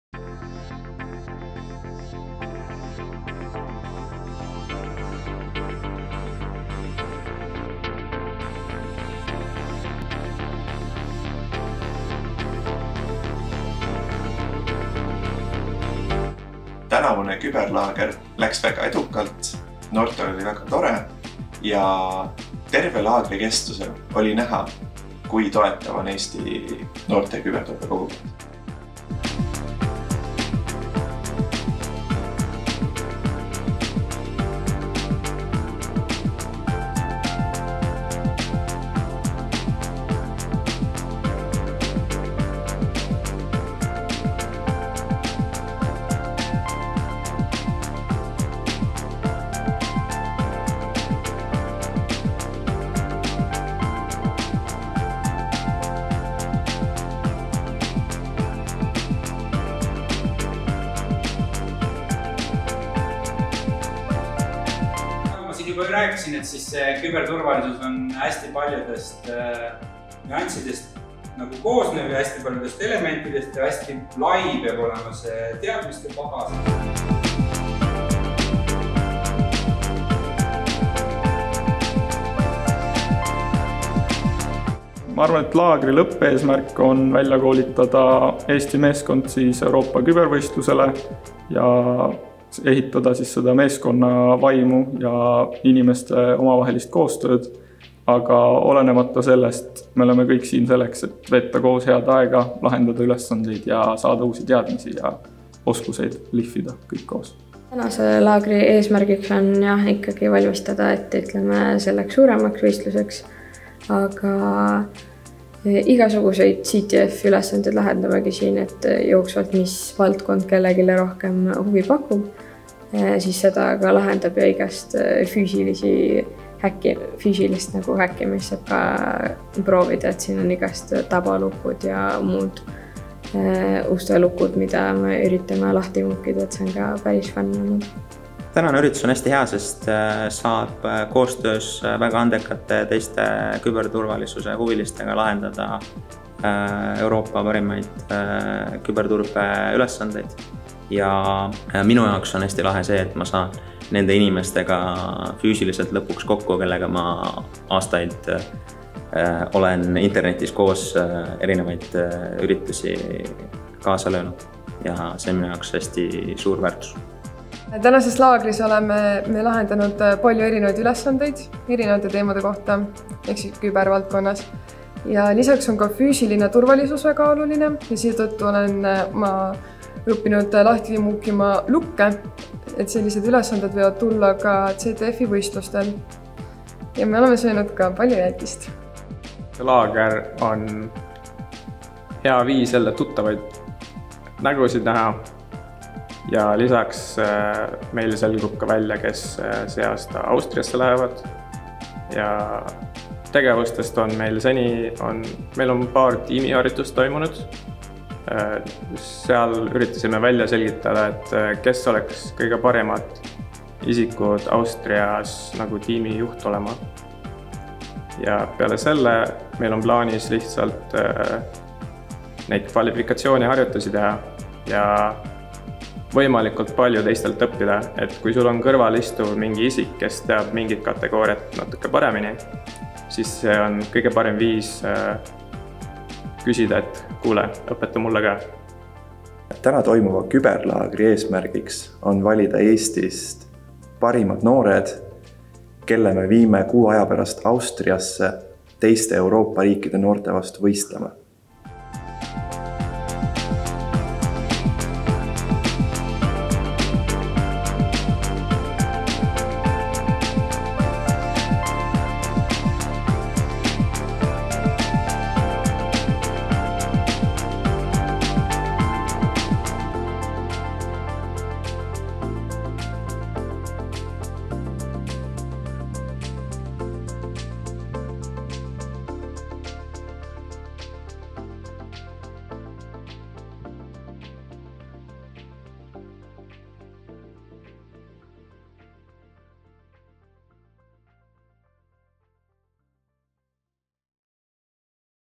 Eesti Küberlaager 2022
Eesti parimad noored kübertalendid osalesid 2022. aasta augustis Eesti Interneti KüberLaagris. Osalejad kuulasid nii teooriat kui ka lahendasid praktilisi ülesandeid, et end rahvusvaheliseks võistluseks parimal moel ette valmistada.

Tegevusi talentidega toetatakse Eesti Interneti SA projektist “Eesti küberkaitse talendid Euroopasse ja parimate praktikate jagamine”, mille elluviija on Tallinna Tehnikaülikool.